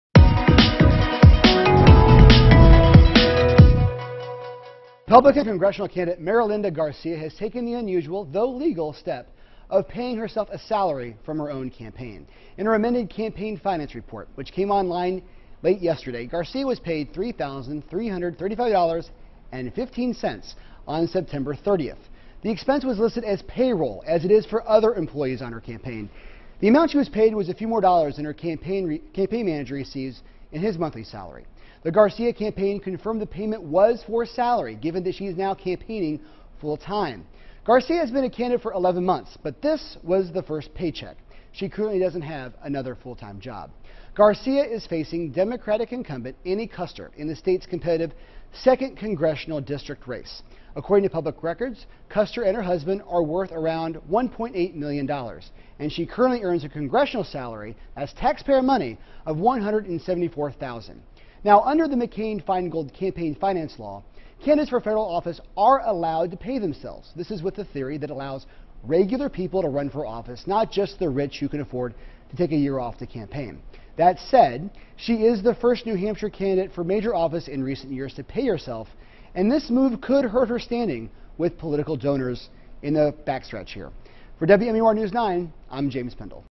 Garcia pays herself salary from campaign
Republican congressional candidate Marilinda Garcia has taken the unusual -- though legal -- step of paying herself a salary from her campaign.